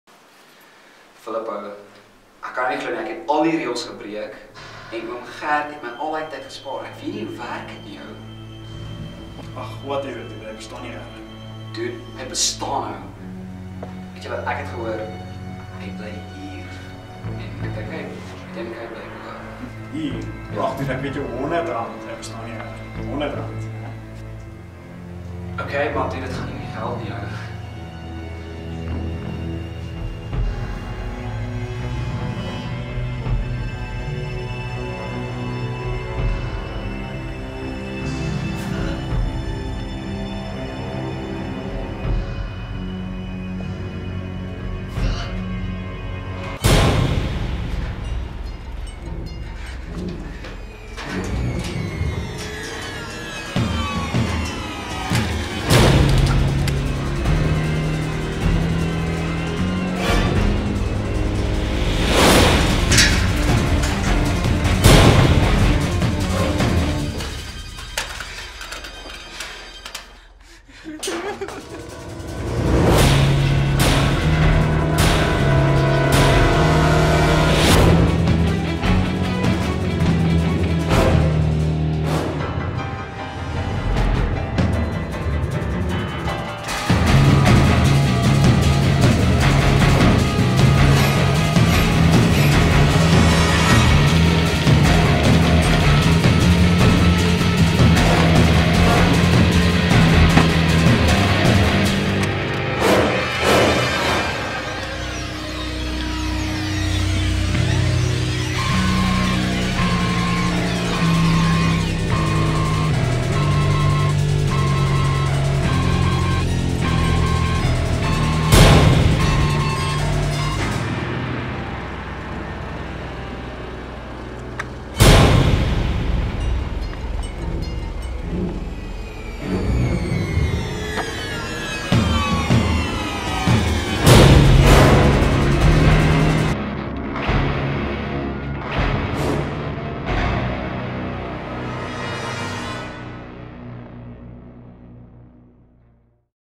Oom Gert
Oom Gert knows where you live and what you did on camp. Phillip finds out the hard way that Oom Gert is after Riaan after he broke every camp rule he could.